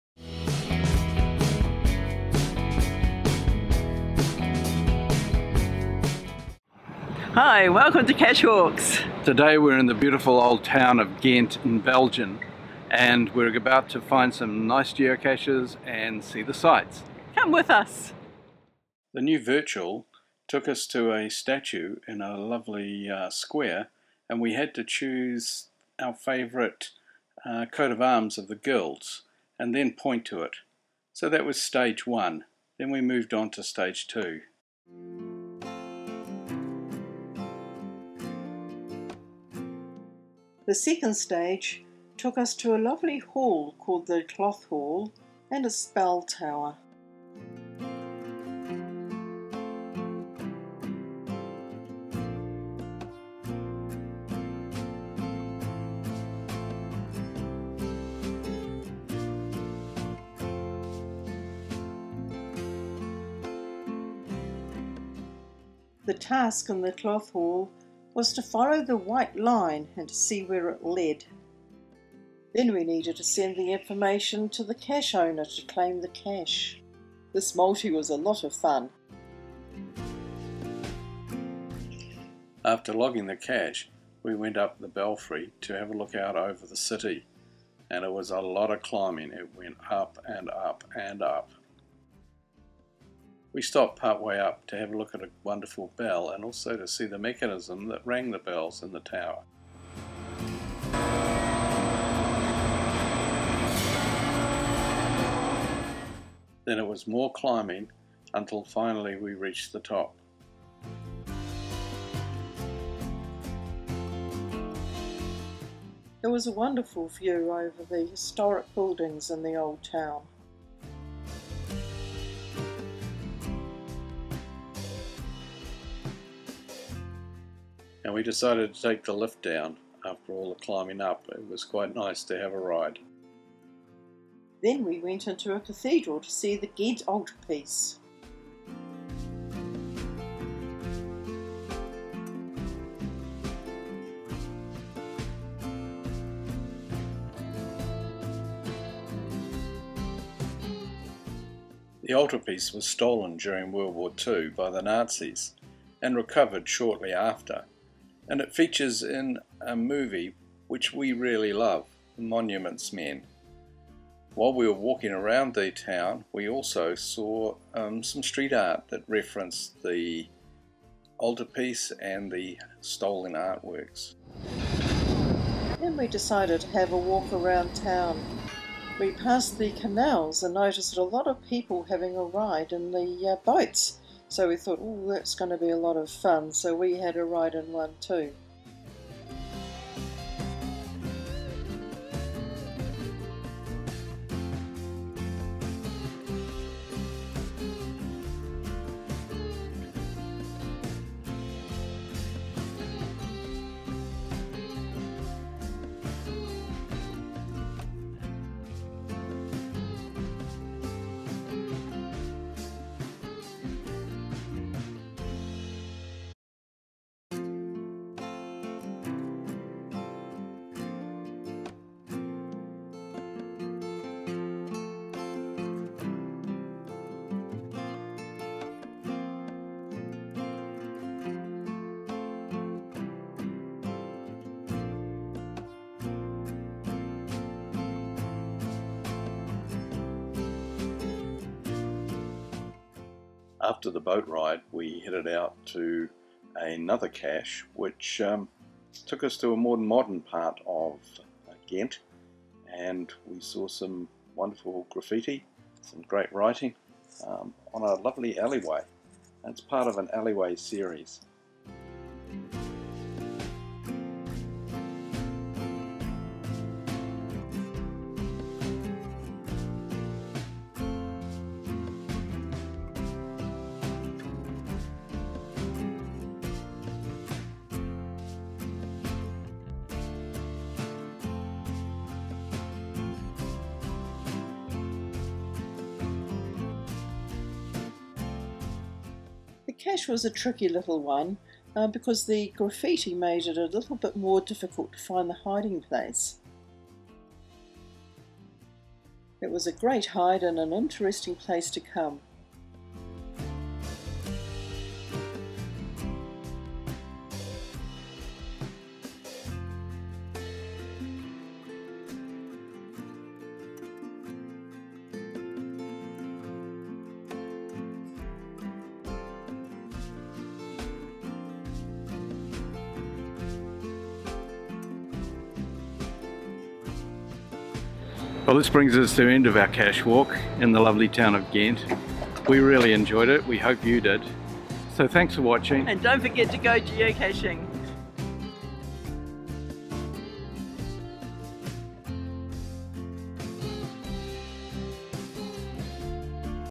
Ghent, Belgium
This Cache Walk is about visiting Ghent, Belgium and the geocaches we found there.

GC7B8GP
GC3F2XC